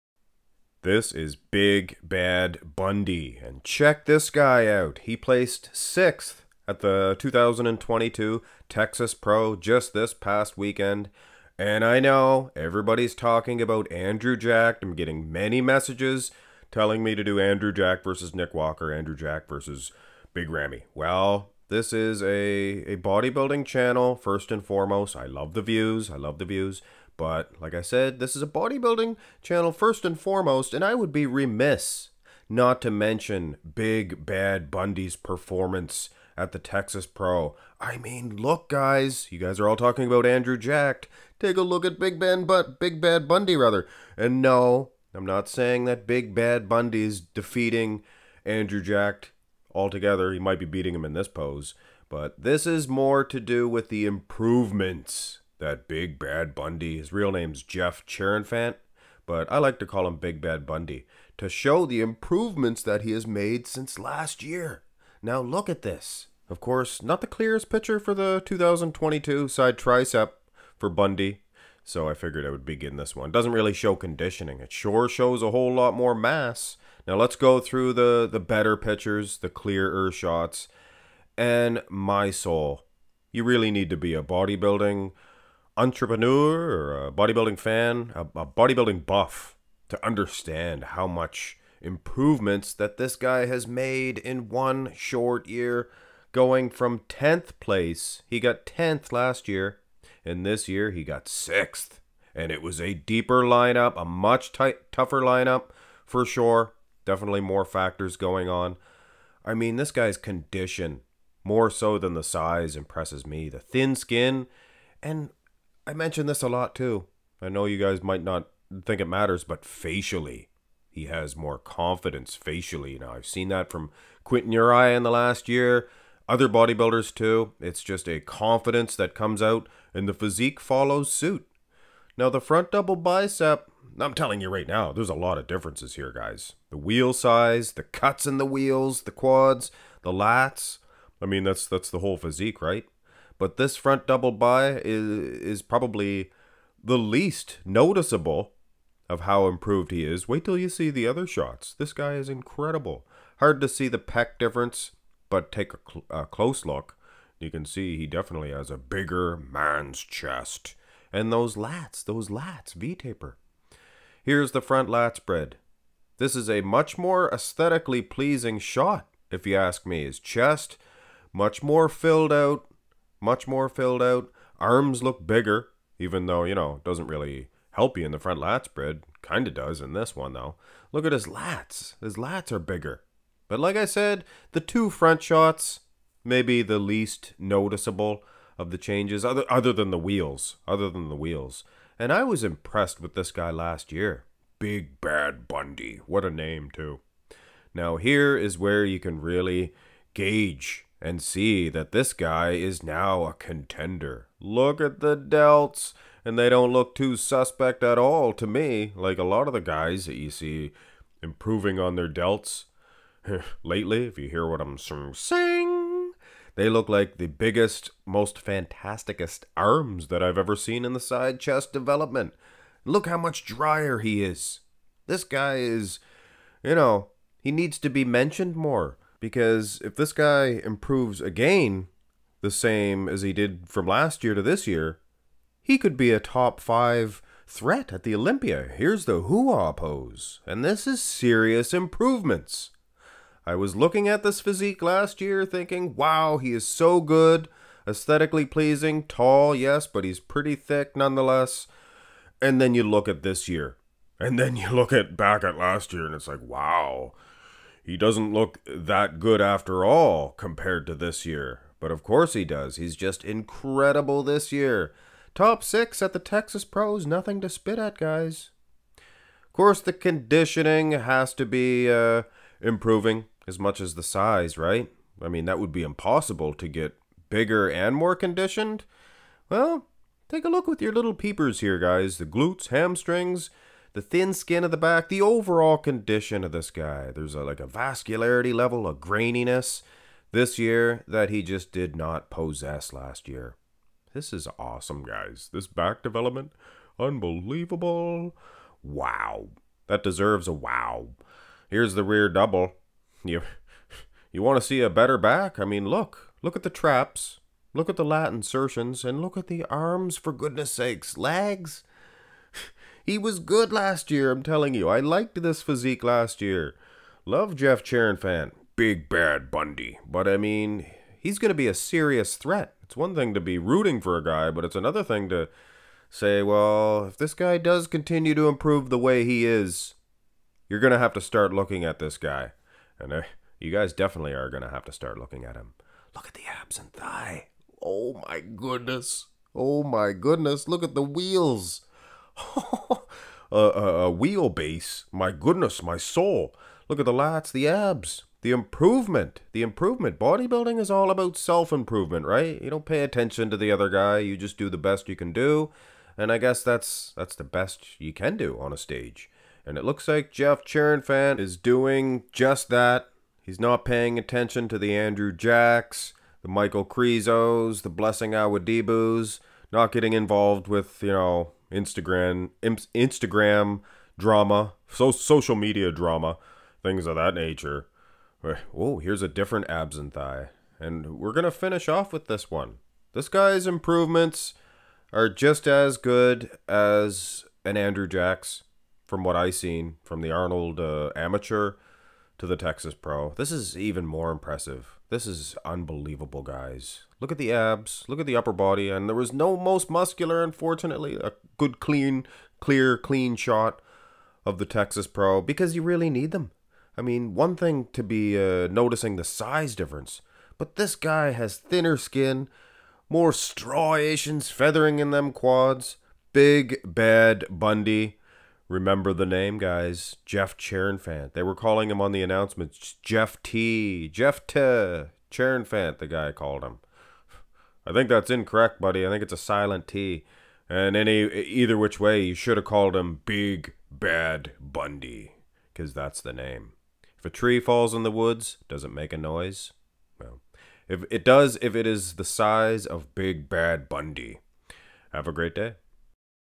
BIG BAD BUNDY Makes a 100% Improvement from Last Year
This is a picture comparison to show the massive improvements Jephte Cherenfant(BIG BAD BUNDY) has made from last years Tampa Pro to this years Texas Pro.